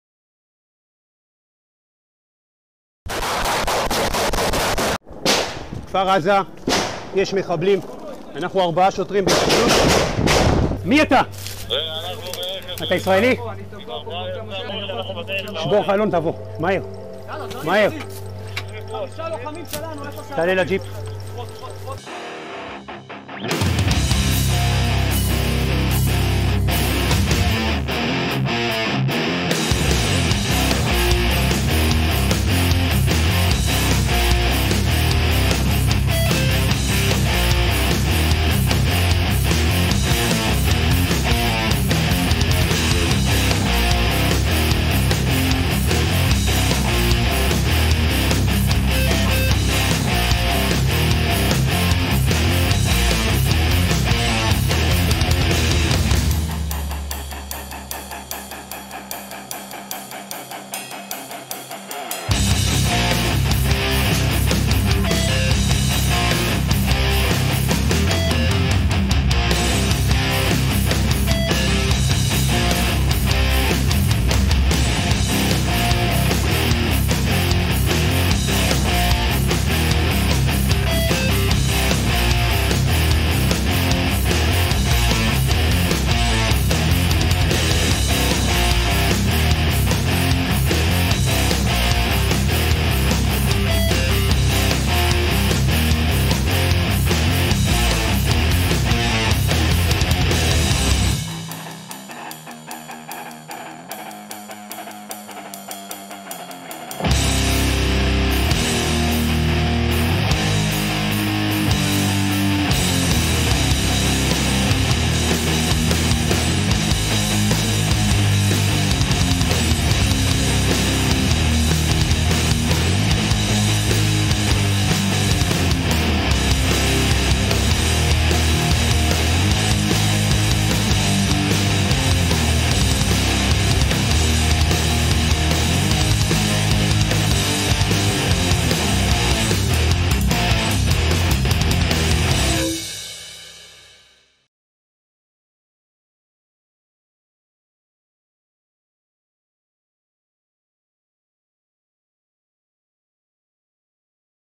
Hero Israeli police officer takes on Hamas terrorists with only a pistol during attack
Dramatic bodycam footage captured a shootout between terrorists and police in Israel. The video was taken by superintendent Keter Tzoran in Kfar Aza on Saturday. For three and a half hours, officer Tzoran battled the jihadists. But officer Tzoran refused to leave his side, and fought off Islamists trying to capture him. By the time IDF reinforcements arrived, Tzoran had just four bullets left.
He was able to evacuate the wounded civilian and another man hiding in a house. And then got out of there himself, leaving the IDF to take up the fight.